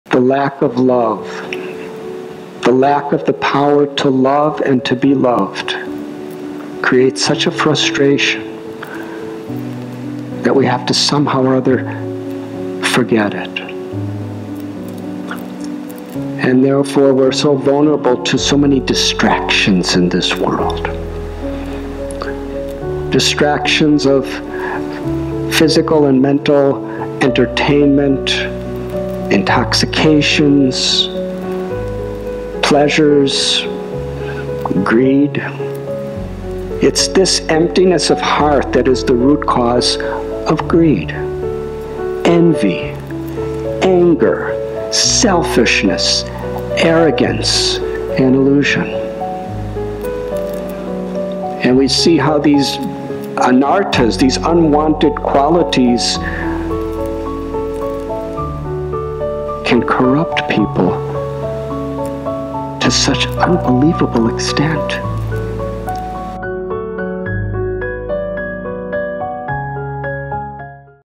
What Lack Of Love Does To Us | His Holiness Radhanath Swami
"To the degree we have love and harmony in our heart, to that degree we can give it to others."- His Holiness Radhanath Swami